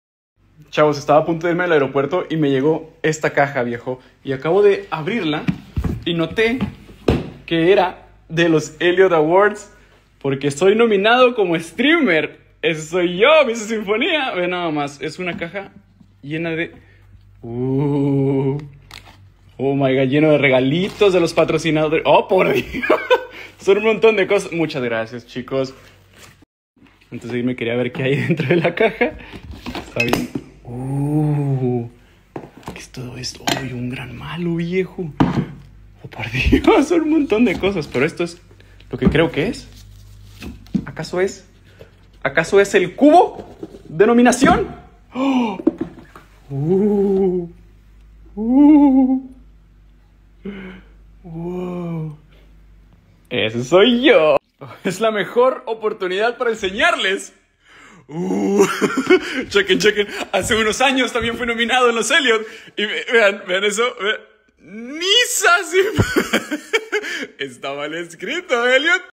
Missasinfonia - Paquete de los Eliot Awards
Historia de Instagram de Missasinfonia subida el 18/10/23

Contexto: missa abre un paquete q le envio el equipo de los Eliot awards